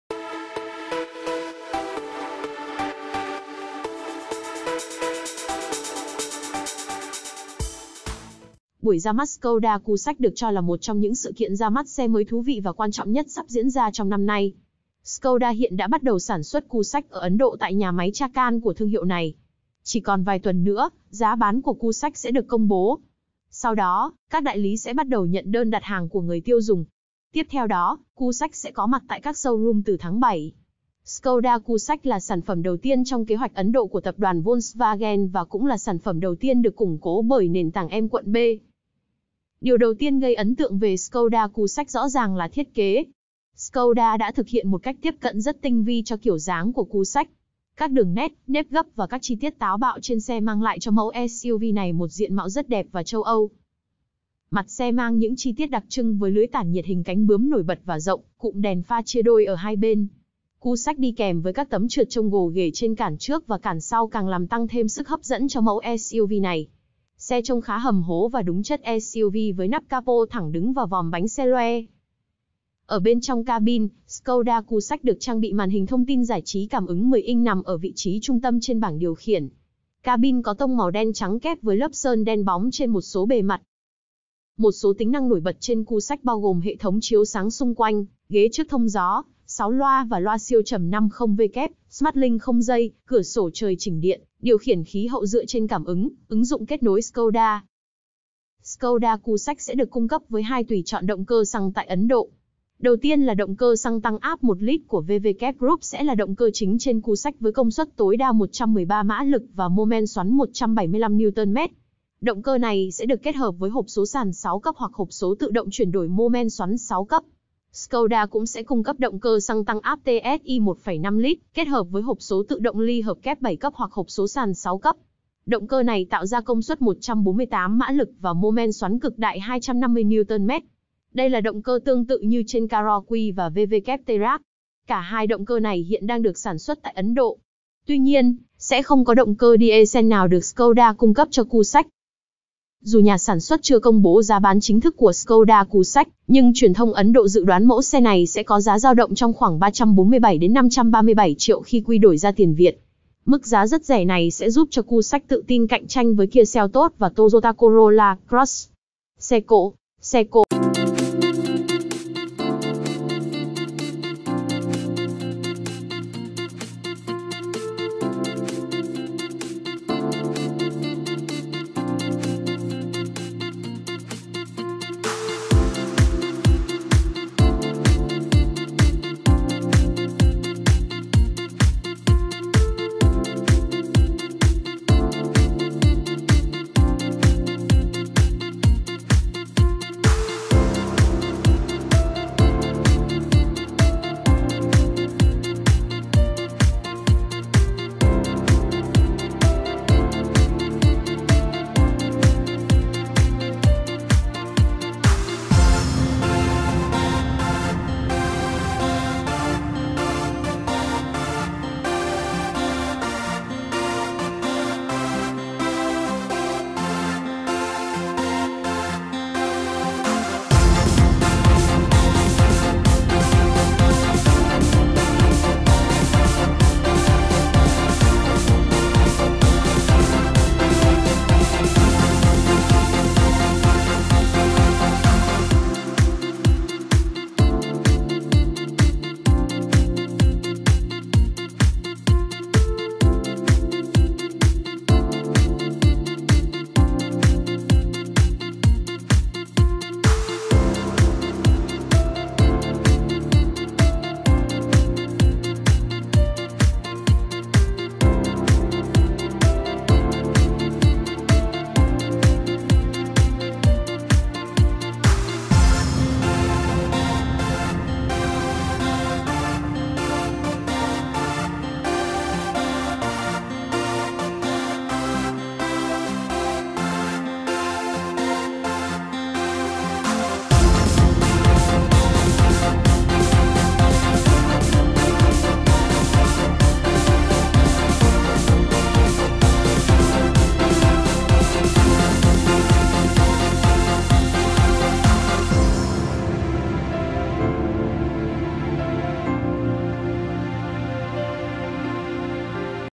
Đấu với KIA Seltos hãng xe sang sắp mở bán SUV giá đúng bèo hơn 500 triệu thiết kế ĐẸP "miễn chê".
Dù nhà sản xuất chưa công bố giá bán chính thức của Skoda Kushaq, nhưng truyền thông Ấn Độ dự đoán mẫu xe này sẽ có giá dao động trong khoảng  347-537 triệu đồng khi quy đổi ra tiền Việt. Mức giá rất rẻ này sẽ giúp cho Kushaq tự tin cạnh tranh với KIA Seltos và Toyota Corolla Cross.